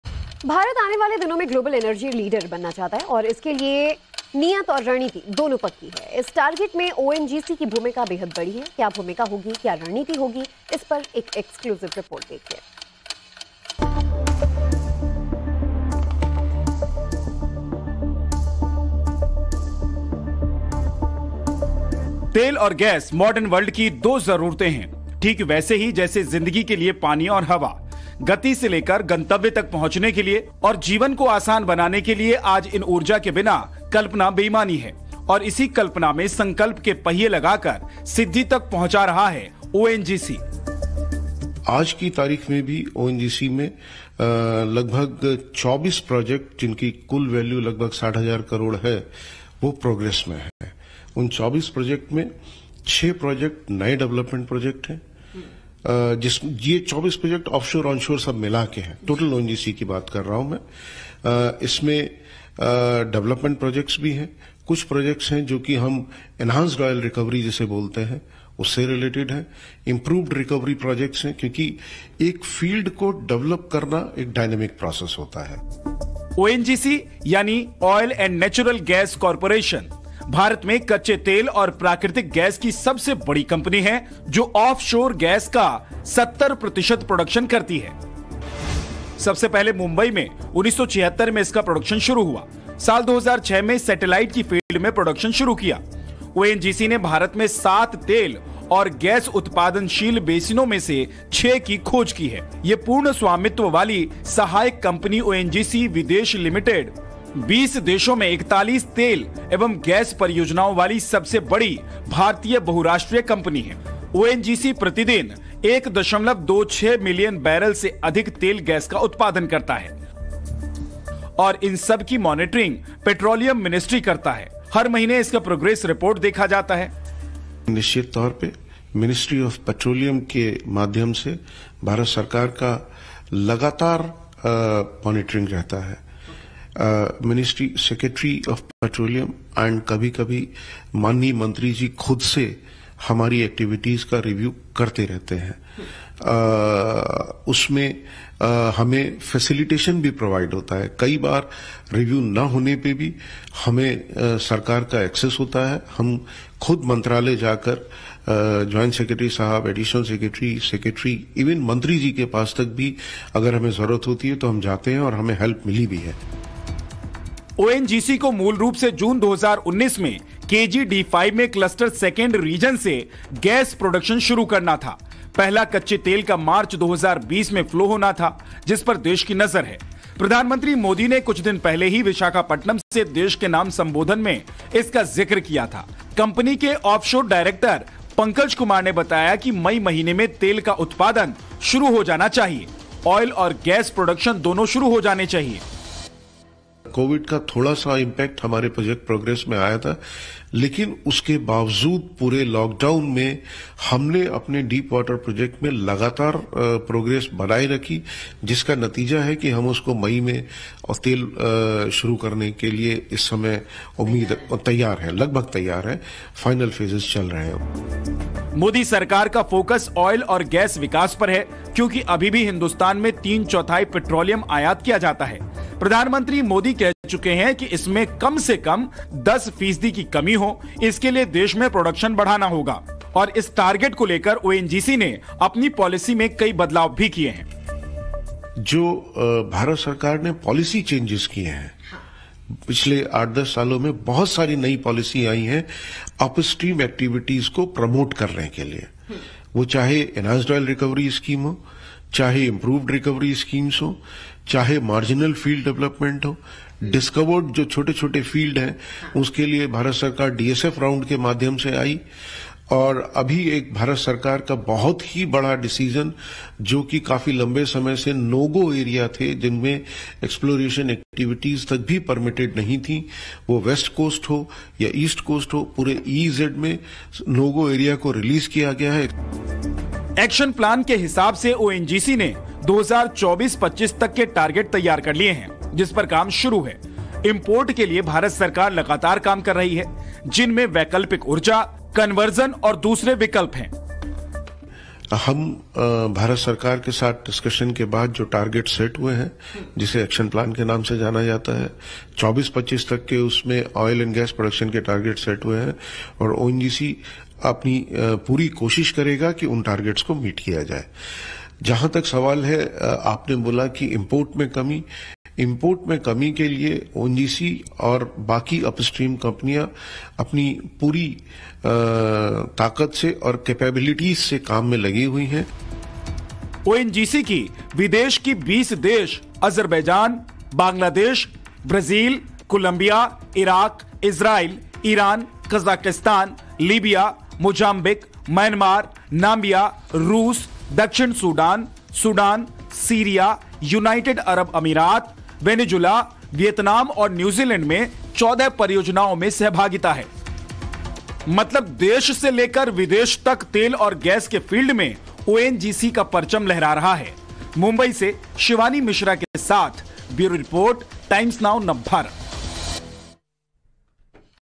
2024 तक ONGC क्या-क्या करने वाला है? | Hindi News
तेल और प्राकृतिक गैस निगम (ONGC), भारत की सबसे बड़ी कच्चे तेल और प्राकृतिक गैस कंपनी, ने देश भर में भू-तापीय ऊर्जा स्रोतों का मानचित्रण करने की योजना की घोषणा की है। योजनाएं अभी भी प्रारंभिक चरण में हैं और लगभग एक वर्ष में क्रियान्वित की जानी चाहिए।